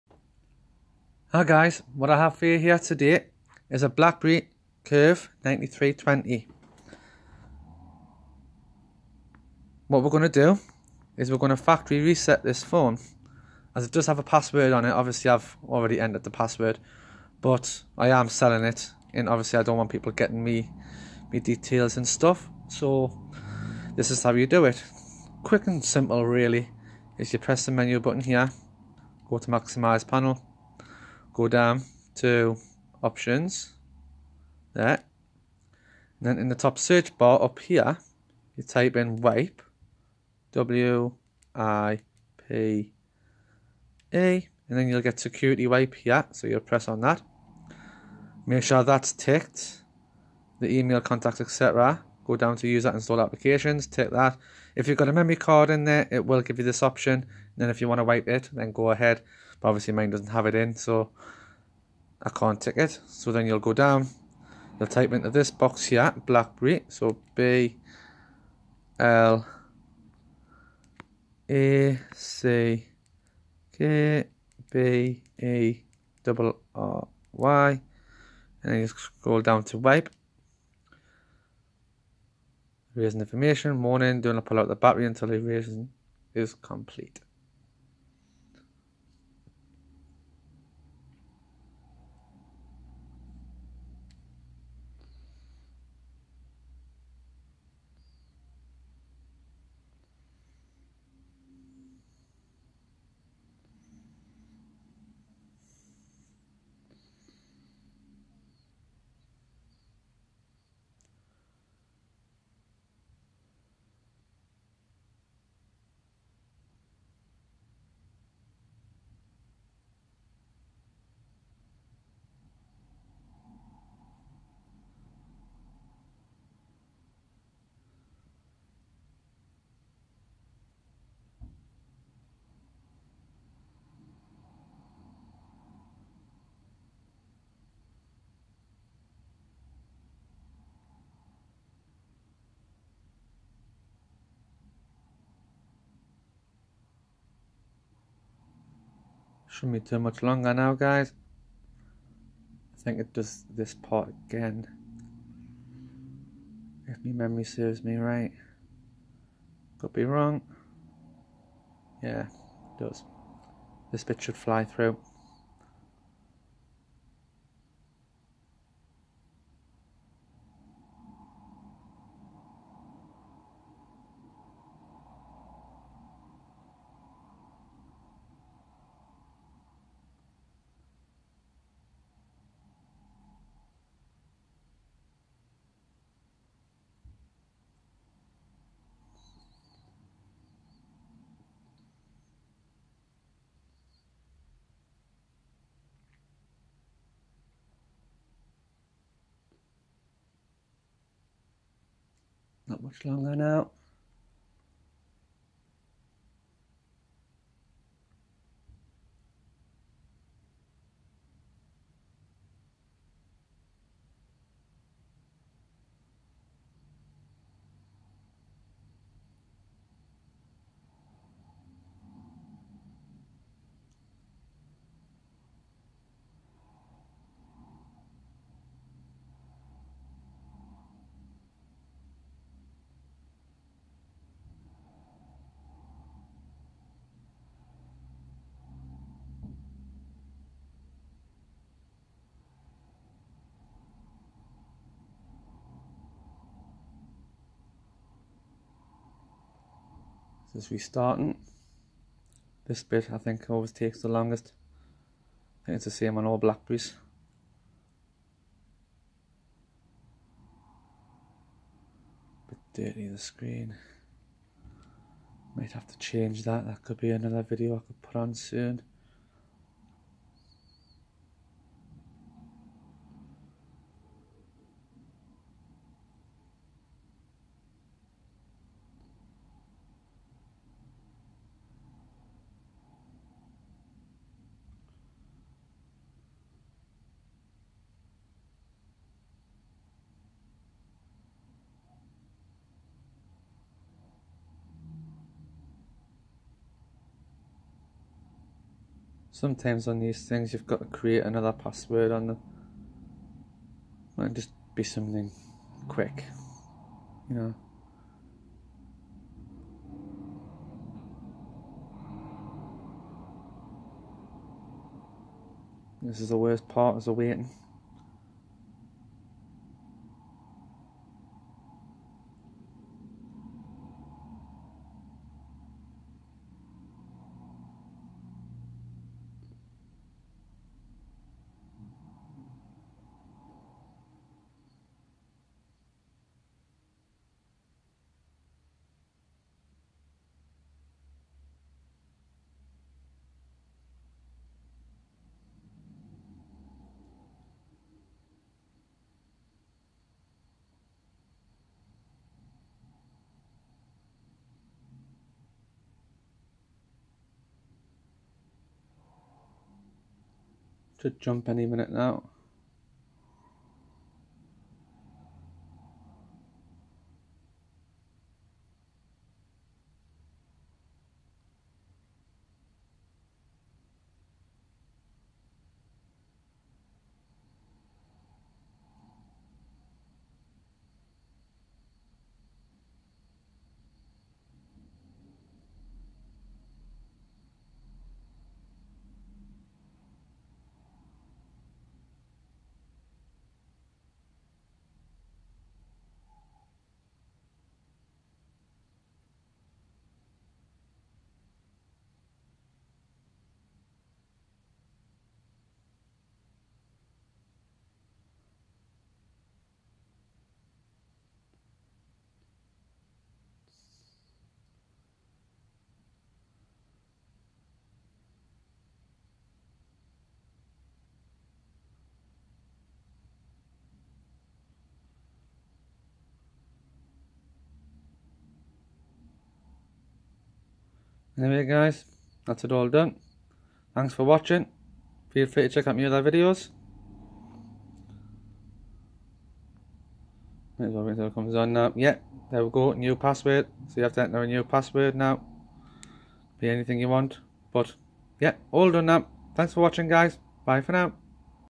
How to wipe/factory reset a BlackBerry curve 9320 easy
Wanting to sell your old phone without all your personal data?
This is what to do.